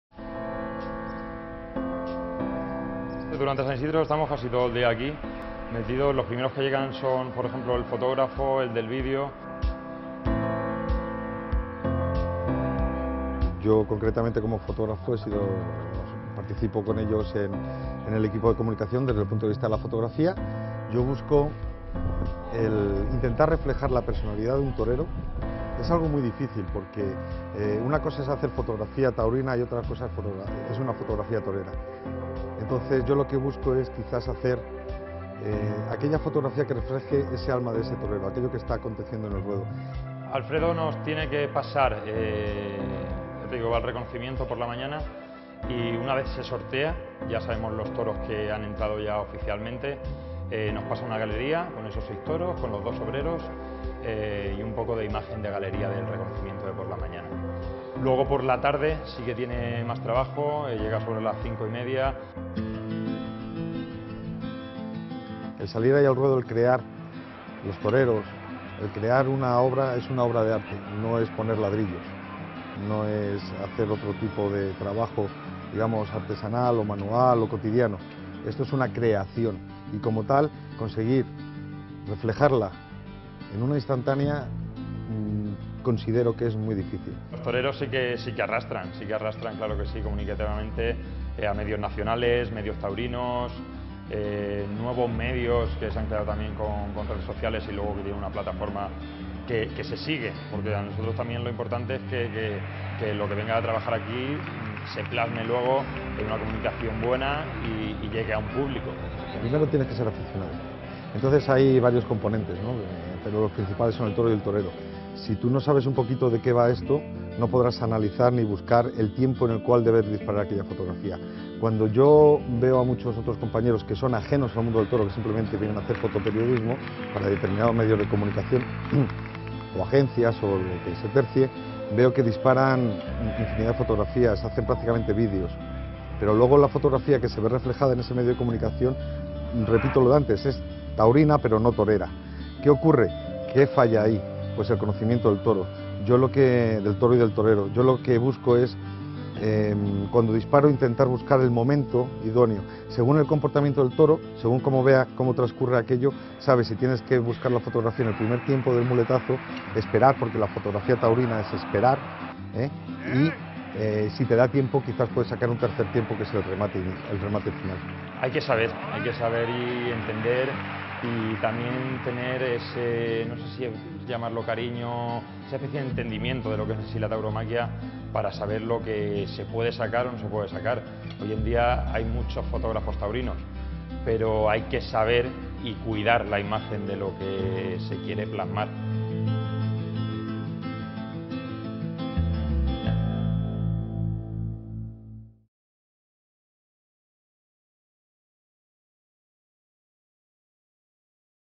Entrevista Alfredo Arévalo. Fotógrafo plaza de las Ventas
Entrevista de "movistar +" a Alfredo Arévalo. Co fundador de Instinto Photo Experience y fotógrafo oficial de Plaza 1 (pl. toros de las Ventas). Junio 2019